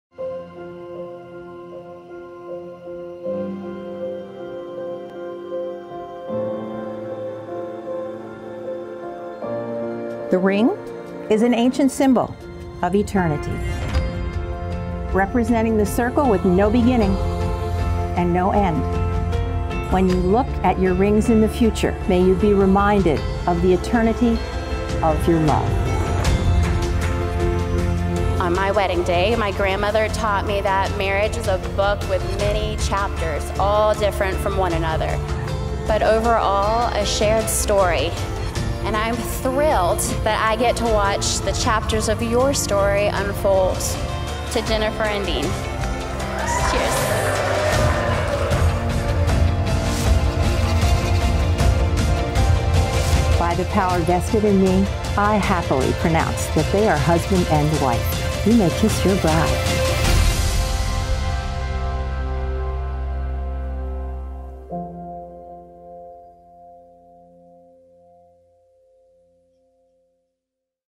Jennifer + Dean's Four Peaks Vermont Wedding Film Teaser
Been on Tik Tok a little too long today? How about checking out a gorgeous autumn wedding in Vermont? Jennifer + Dean tied the knot at the beautiful Four Peaks in Weston, VT last October.

Wedding Film: Sixpence Productions
Venue: Four Peaks, Weston, Vermont
Officiant: Annmarie Alexander-Kramer
Photography: Anne-Marie Photography⠀⠀⠀⠀⠀⠀⠀
Wedding Dress: Emma & Grace Bridal Studio⠀⠀⠀⠀⠀⠀⠀⠀⠀
Wedding Planner: Evelyn Makes Magic
Florist: Hybl Fannin Design
Makeup: Jennifer Perellie Makeup
Hair: Misty Haskell
Caterer: Vermont Farms Catering
Bridal Party Dresses: The Dessy Group